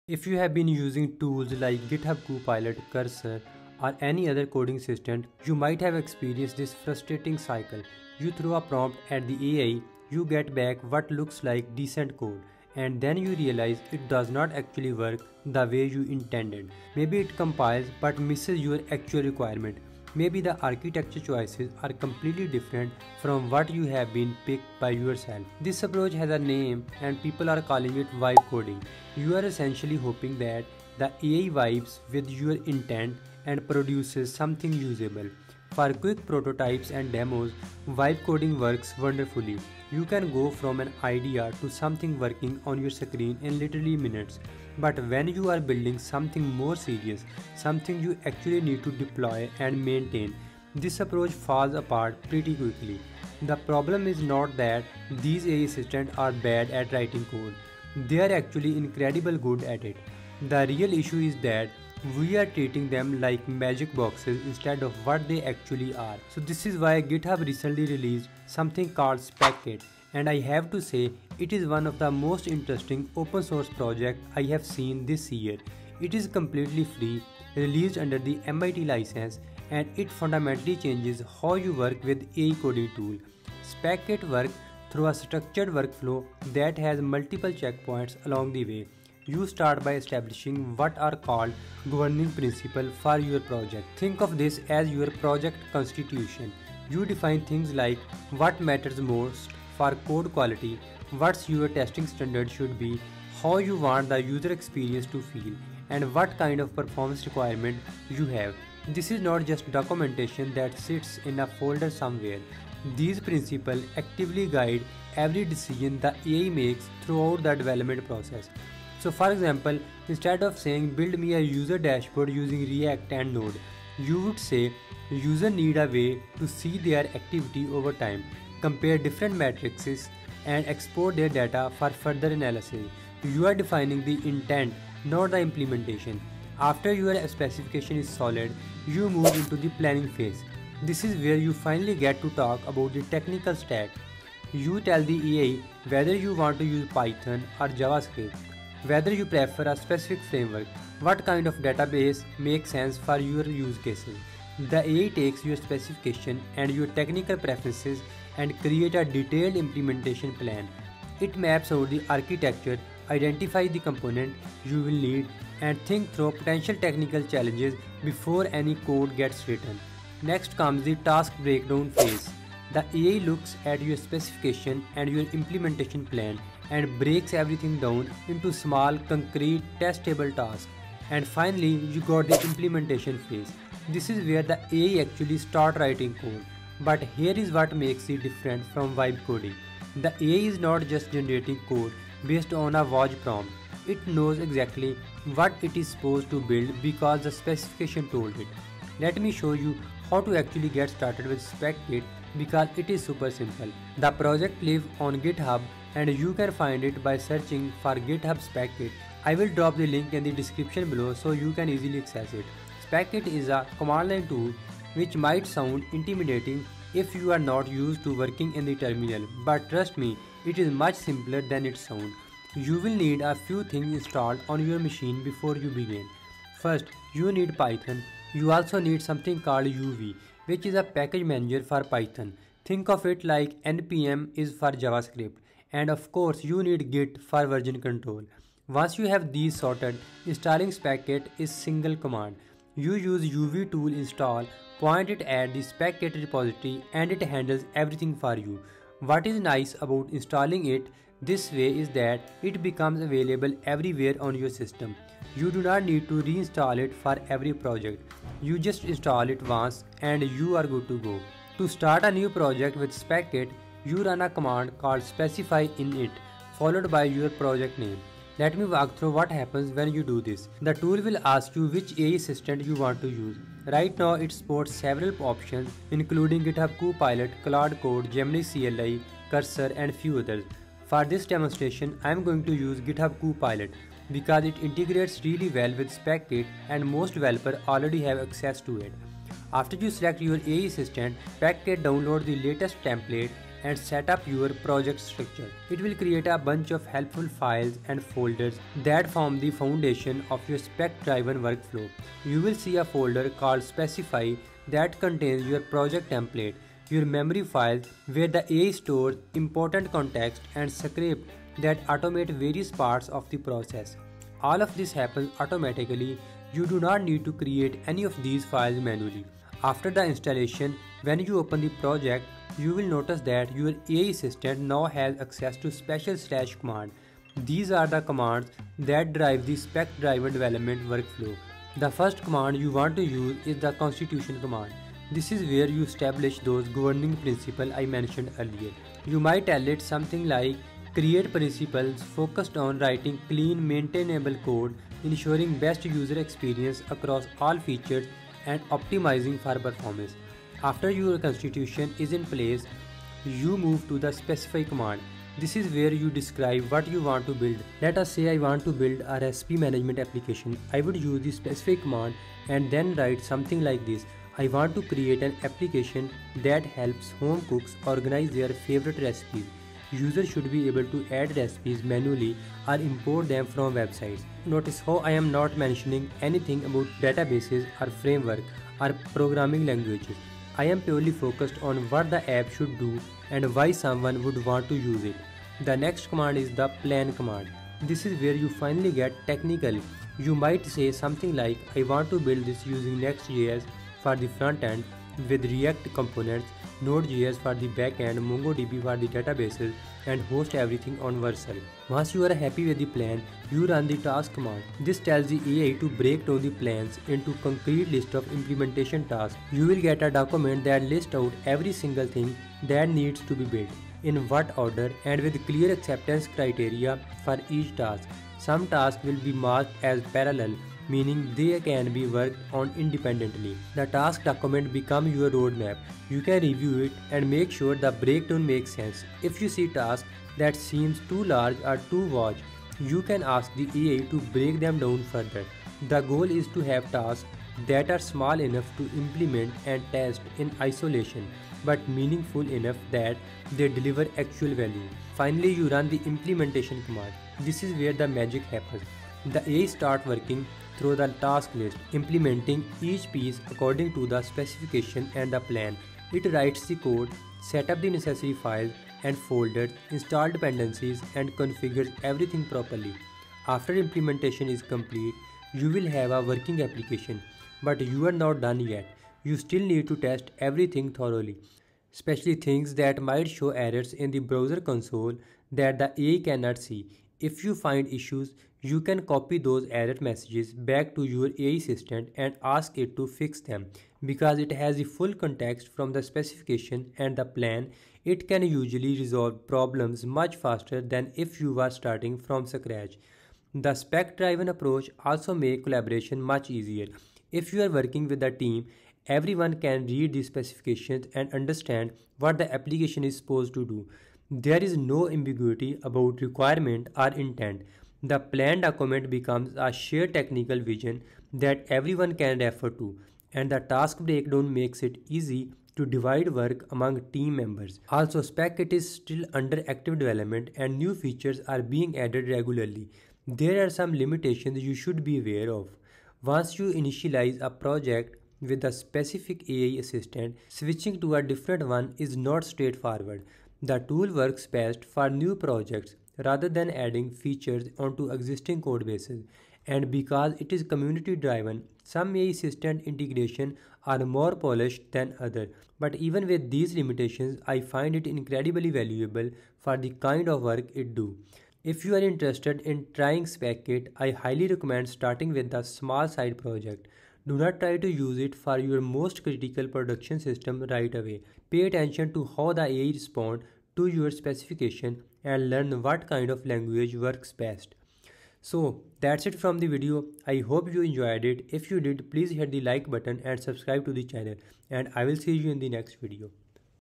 GitHub SpecKit DESTROYS Vibe Coding - Build Better Apps Using Spec-Driven Development
Stop throwing prompts at AI and hoping for the best. GitHub just released SpecKit, an open-source toolkit that completely destroys vibe coding and introduces spec-driven development.